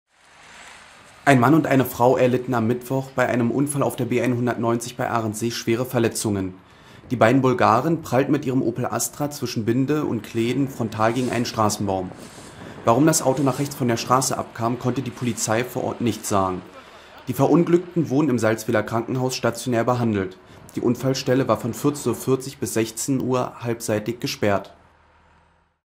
Frontal gegen Baum: Zwei Schwerverletzt auf B 190 bei Arendsee
Zwei Bulgaren erlitten am Mittwochnachmittag bei einem Unfall auf der B 190 zwischen Binde und Kläden schwere Verletzungen. Wie die Polizei vor Ort mitteilte, kam der Opel, der in Richtung Arendsee fuhr, aus bisher ungeklärter Ursache nach rechts von der Straße ab und prallte mit voller Wucht frontal gegen einen Straßen Baum.